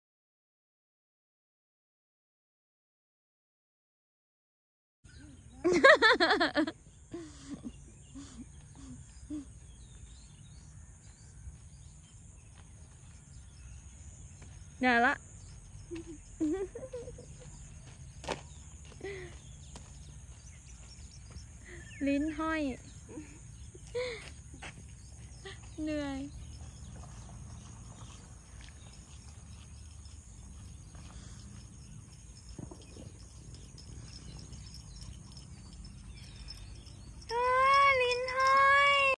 เมื่อสุนัข Indor ออกสำรวจชายทุ่ง
อยู่บ้านทั้งวัน ต้องออกมาสำรวจนอกบ้านบ้าง แดดอาจจะร้อนสักหน่อย แต่สนุกจนลืมร้อนไปเลย เตรียมผ้าเย็นและน้ำดื่มไปด้วยนะ